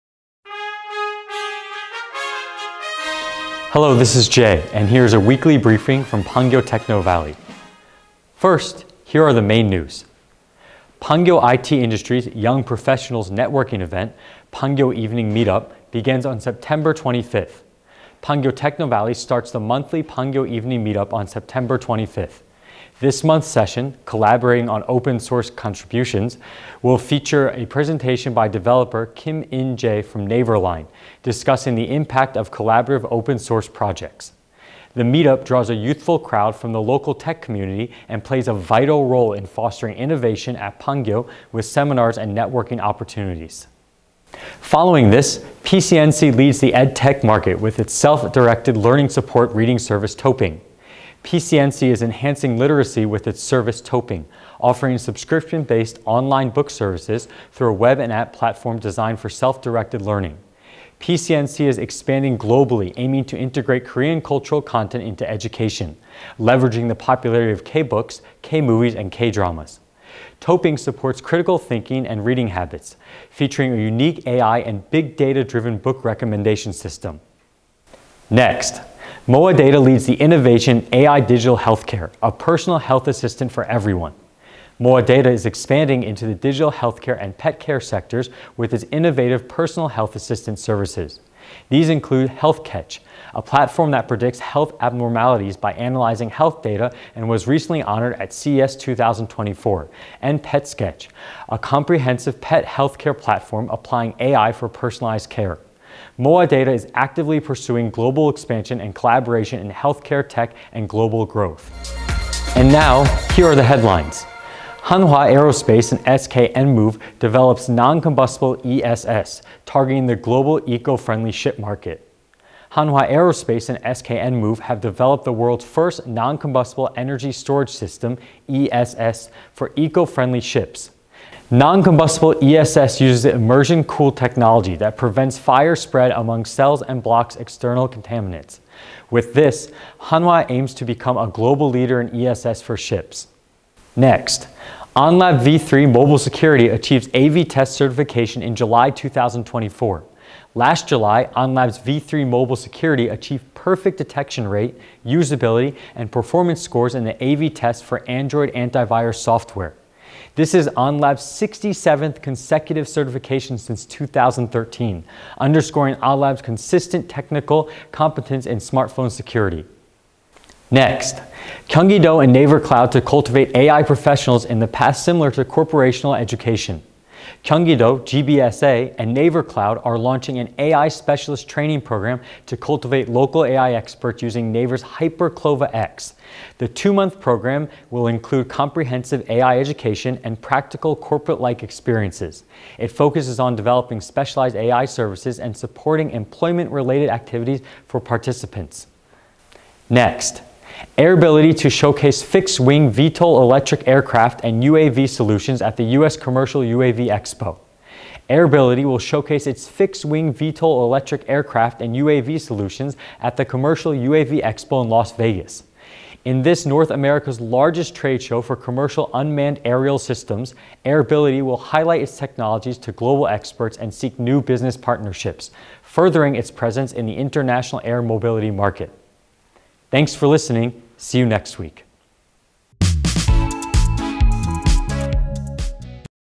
Pangyo Techno Valley Weekly News : Startup & ICT News From Pangyo [20240923]
First, here are the main news.

Pangyo IT Industry’s Young Professionals Networking Event ‘Pangyo Evening Meetup’ Begins on September 25th
Pangyo Techno Valley starts the monthly Pangyo Evening Meetup on September 25th. This month's session, "Collaborating on Open Source Contributions," will feature a presentation by developer Kim In-je from NAVER LINE discussing the impact of collaborative open-source projects. The meetup draws a youthful crowd from the local tech community and plays a vital role in fostering innovation at Pangyo with seminars and networking opportunities.

PCNC Leads the EdTech Market with its Self-Directed Learning Support Reading Service ‘TOAPING’
PCNC is enhancing literacy with its service, 'TOPPING,' offering subscription-based online book services through a web and app platform designed for self-directed learning. PCNC is expanding globally, aiming to integrate Korean cultural content into education, leveraging the popularity of K-books, K-movies, and K-dramas. TOAPING supports critical thinking and reading habits, featuring a unique AI and big data-driven book recommendation system.

MOAdata Leads the Innovation in AI Digital Healthcare: ‘A Personal Health Assistant’ for Everyone
MOAdata is expanding into the digital healthcare and pet care sectors with its innovative 'Personal Health Assistant' services. These include Healthketch, a platform that predicts health abnormalities by analyzing health data and was recently honored at CES 2024, and Petsketch, a comprehensive pet healthcare platform applying AI for personalized care. MOAdata is actively pursuing global expansion and collaboration in healthcare tech and global growth.

And now, here are the headlines.

Hanwha Aerospace & SK Enmove Develops ‘Non-Combustible ESS,’ Targeting the Global Eco-Friendly Ship Market
Hanwha Aerospace and SK Enmove have developed the world's first 'non-combustible energy storage system (ESS)' for eco-friendly ships. Non-Combustible ESS uses immersion-cooled technology that prevents fire spread among cells and blocks external contaminants. With this, Hanwha aims to become a global leader in ESS for ships.

AhnLab V3 Mobile Security Achieves ‘AV-TEST’ Certification in July 2024
Last July, AhnLab's V3 Mobile Security achieved perfect detection rate, usability, and performance scores in the AV-TEST for Android antivirus software. This is AhnLab's 67th consecutive certification since 2013, underscoring AhnLab’s consistent technical competence in smartphone security.

Gyeonggi-do and Naver Cloud to Cultivate AI Professionals, in the Path Similar to Corporational Education
Gyeonggi-do, GBSA, and Naver Cloud are launching an AI Specialist Training Program to cultivate local AI experts using Naver’s Hyper CLOVA X. The two-month program will include comprehensive AI education and practical corporate-like experiences. It focuses on developing specialized AI services and supporting employment-related activities for participants.

AIRBILITY to Showcase Fixed-Wing VTOL Electric Aircraft and UAV Solutions at the U.S. Commercial UAV Expo
AIRBILITY will showcase its fixed-wing VTOL electric aircraft and UAV solutions at the Commercial UAV Expo in Las Vegas. In this North America's largest trade show for commercial Unmanned Aerial Systems, AIRBILITY will highlight its technologies to global experts and seek new business partnerships, furthering its presence in the international air mobility market.